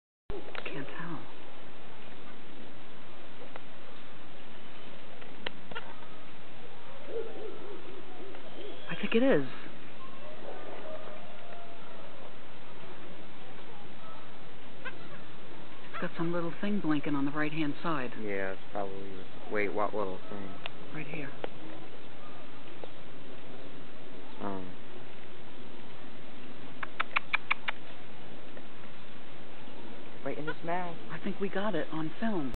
Monkey Catches and Eats Food By Hand
Monkey at Popcorn Park Zoo in New Jersey: This cute monkey knows how to catch food and eat it by my Aunt and I throwing popcorn for him :)